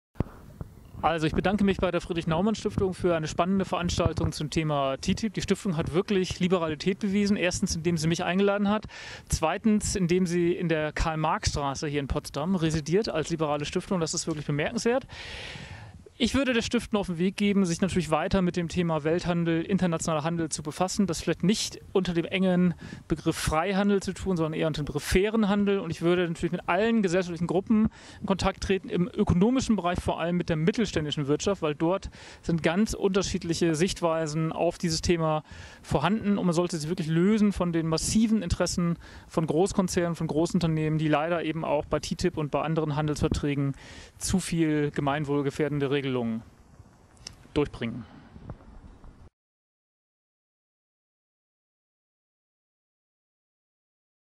Statt TTIP an fairen Welthandel denken!
Wir haben uns mit Dr. Michael Efler, Bundesvorstandsmitglied bei Mehr Demokratie e.V. über TTIP unterhalten. Auch wenn wir uns nicht in allen Punkten einig waren: Vielen Dank für den guten Diskurs!